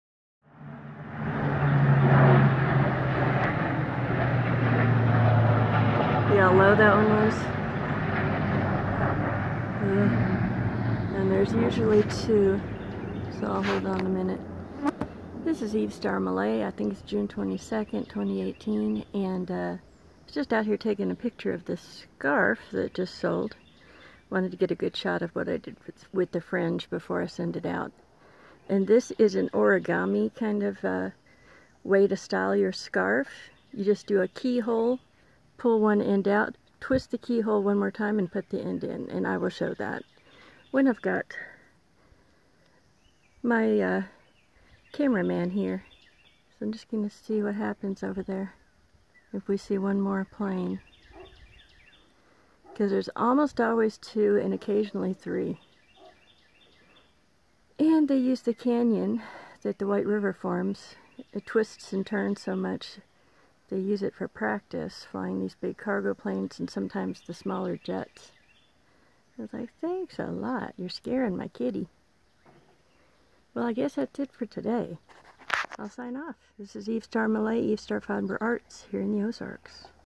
Saori weaving summer scarf wabi sabi upcycled style! Eve Starr fiber arts, hand dyed hand spun art
The Ozark mountain view is perfect for my gaudy bird scarves. Rigid heddle loom with a view. Be inspired by your own surrounding and translate it to fiber art with me! My videos share tips and tutorials on living well with chronic pain to being creative on the cheap.